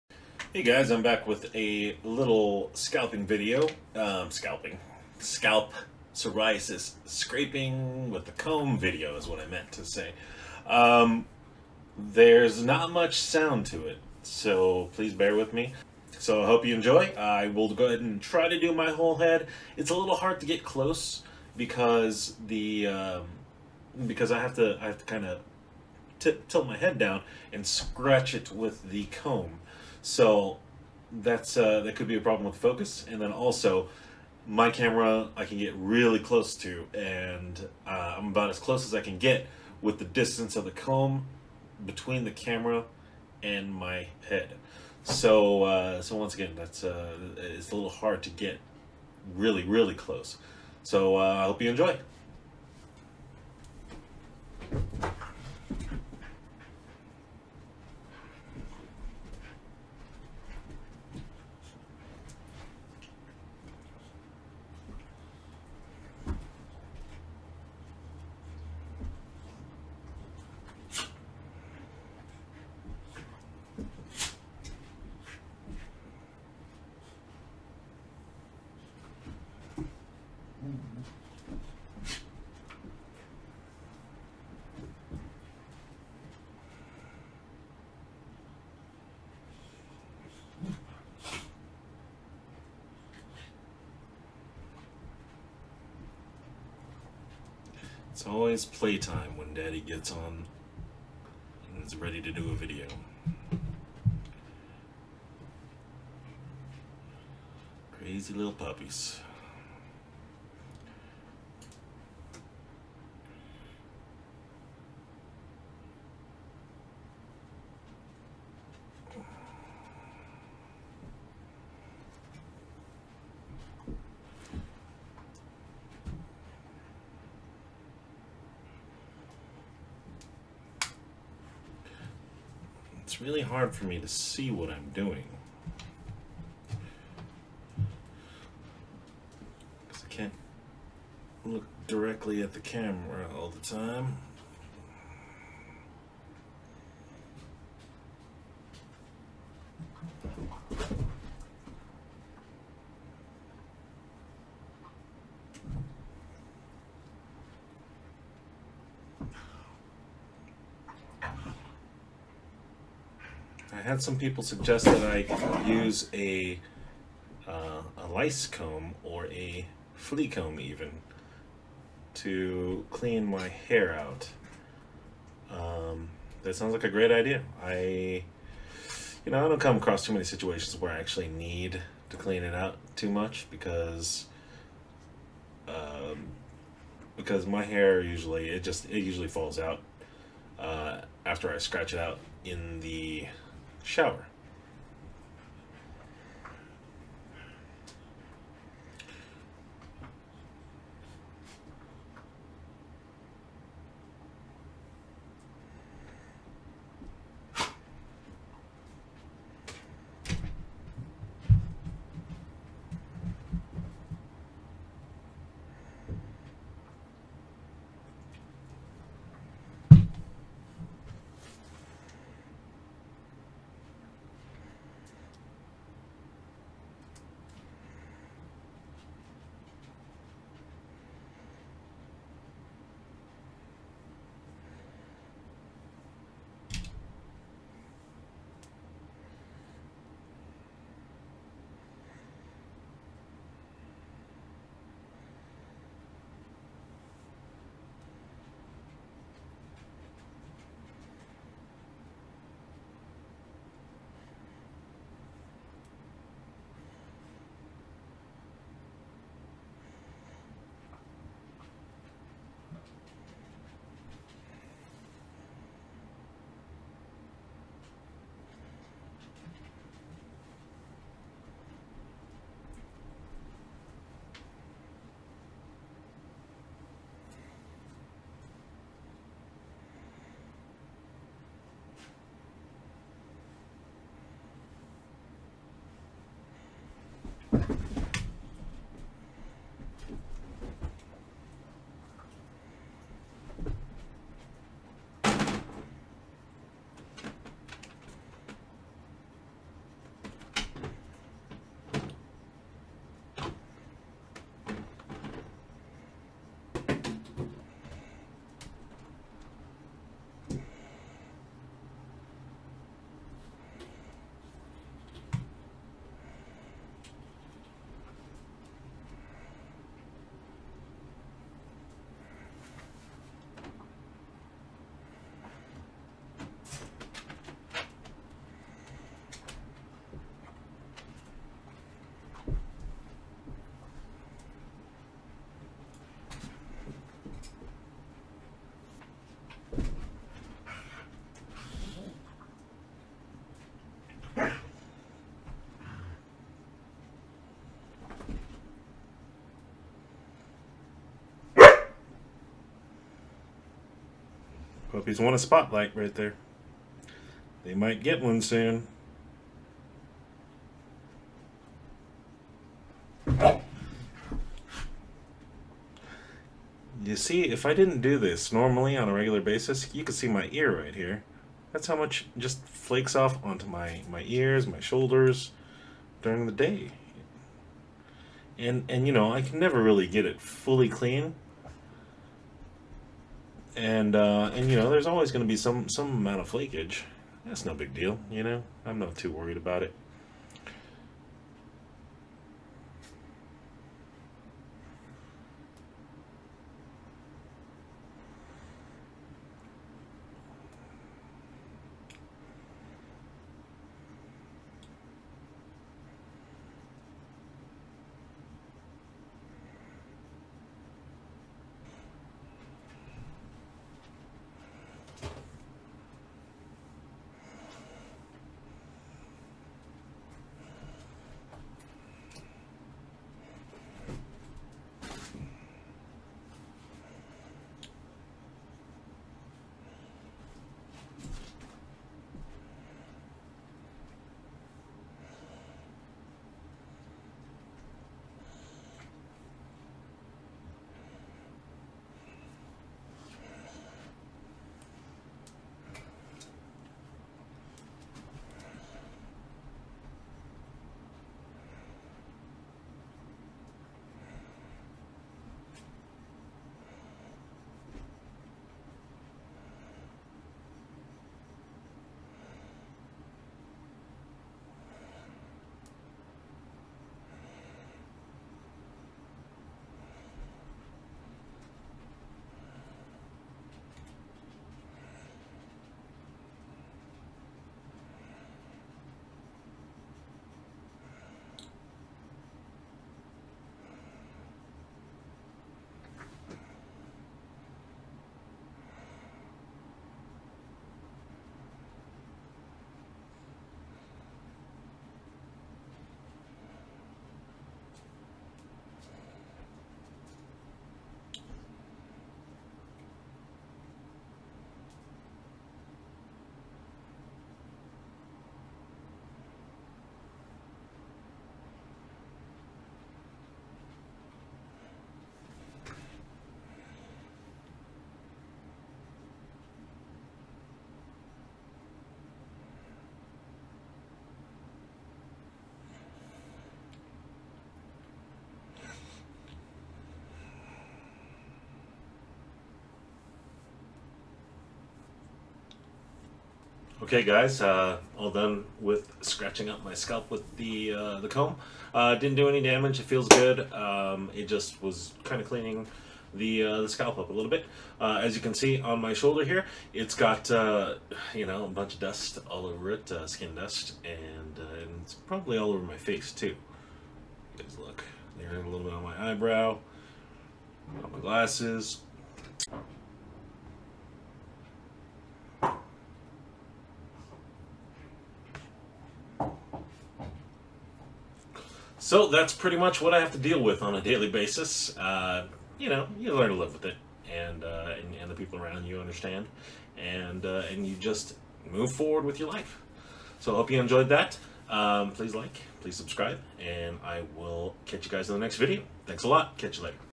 SCALP PSORIASIS FLAKING ALLOVER!
Just cleaning off my scalp with a metal comb!

There are many types of psoriasis:
plaque
guttate
inverse
pustular
erythrodermic
psoriatic arthritis